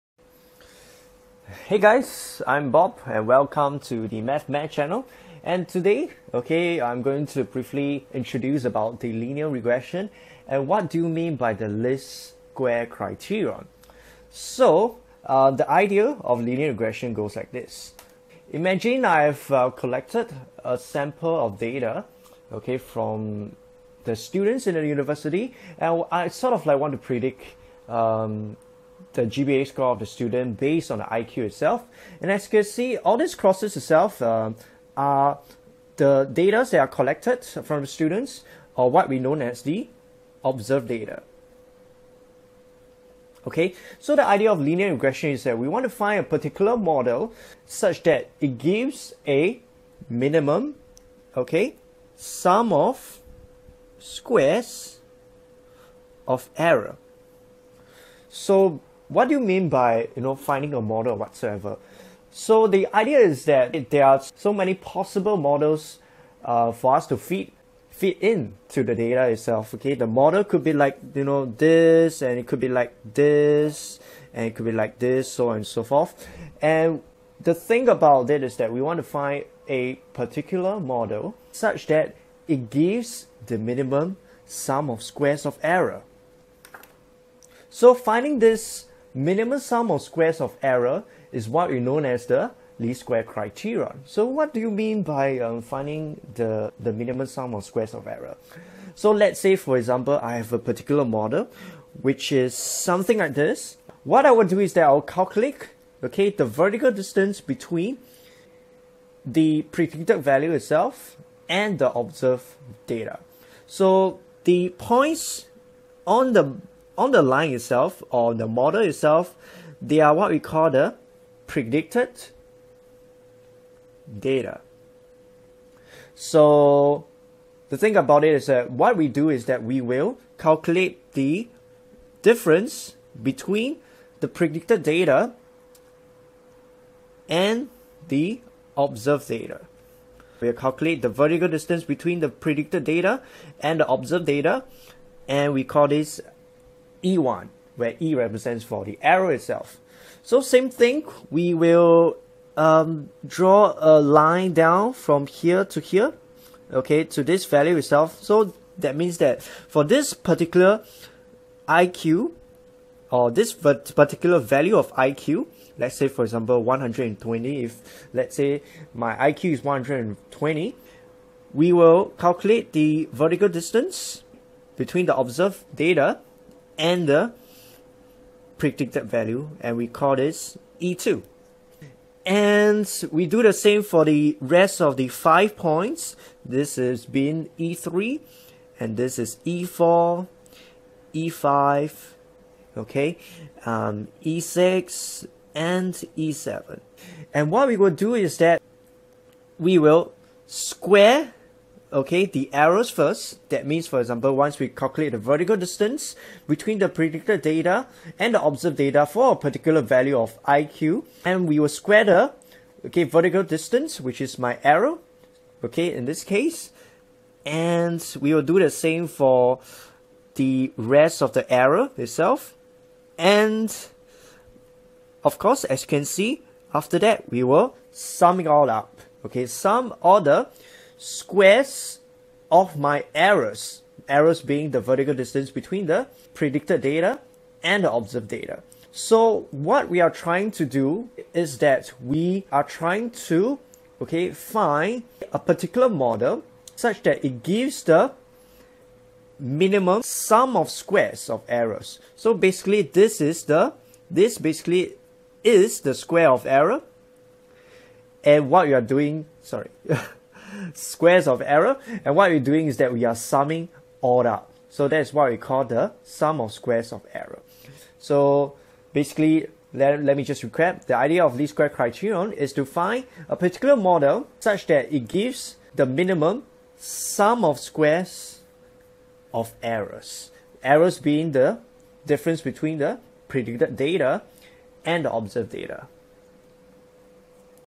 Introduction to Linear Regression - Least Square Criterion | PART 1
Here's a short brief video about the least square criterion (Ordinary Least Square Method) used in the simple linear regression. :)